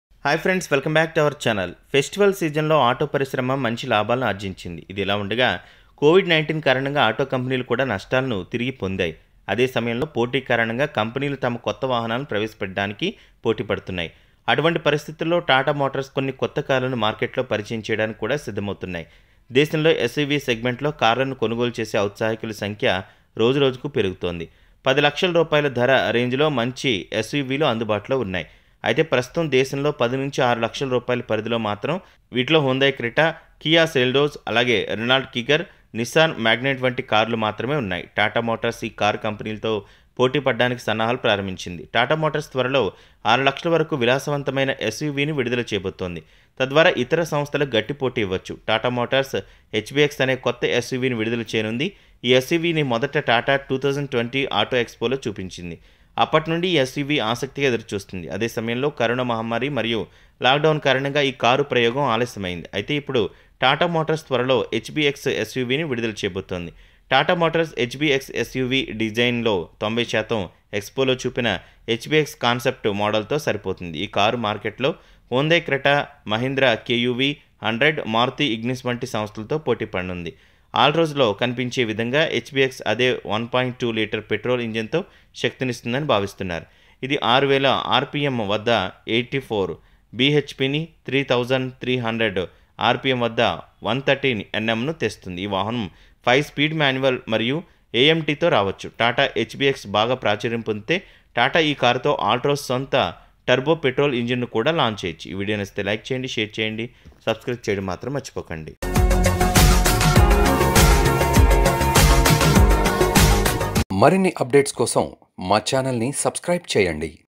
4.44 లక్షల అతి తక్కువ ధరలో మధ్యతరగతి కుటుంబాల కోసం 38kmplతో మార్కెట్లోకి వచ్చేసిన TATA HBX
Watch : 4.44 లక్షల అతి తక్కువ ధరలో మధ్యతరగతి కుటుంబాల కోసం 38kmplతో మార్కెట్లోకి వచ్చేసిన TATA HBX